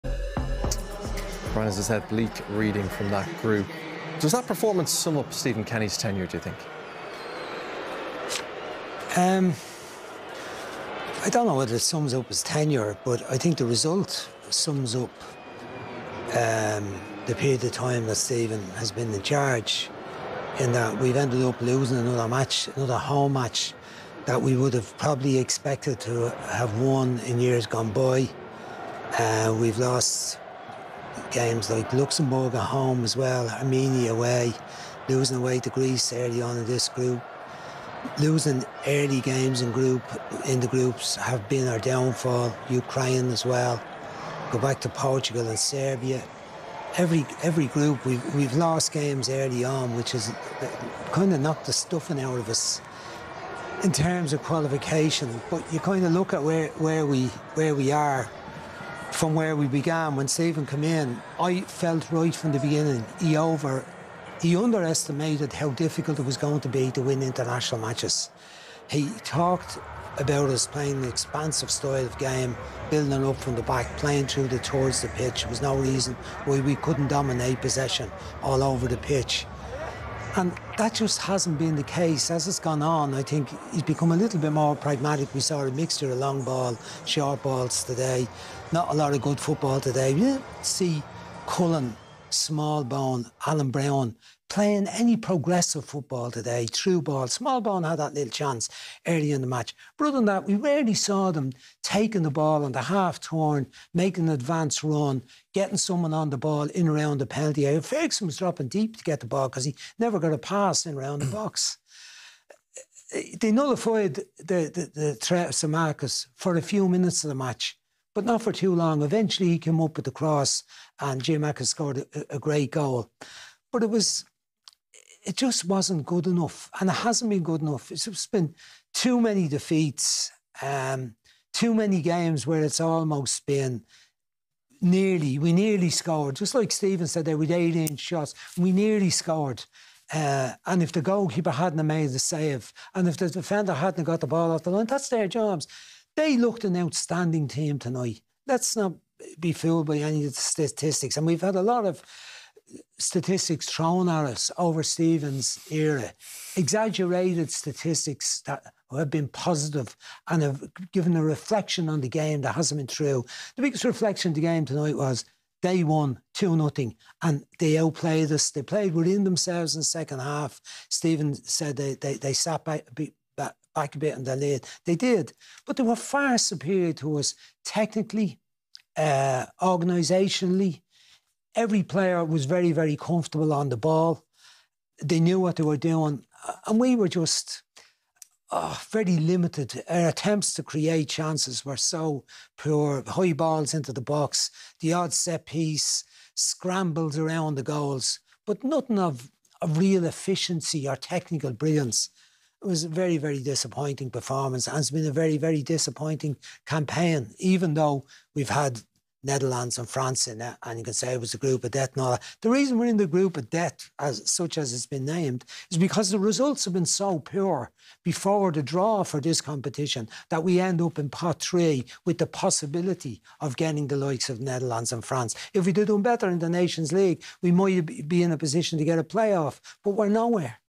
Brian Kerr reviews Stephen Kenny's tenure after defeat to Greece
🗣 "I felt right from the beginning he underestimated how difficult it was going to be to win international matches."

🗣 "We're nowhere."

Was tonight a reflection of the Stephen Kenny tenure as a whole? Brian Kerr has his say.

Watch UEFA Champions League, Europa League, Europa Conference League, Nations League and European Qualifiers live on Virgin Media Television.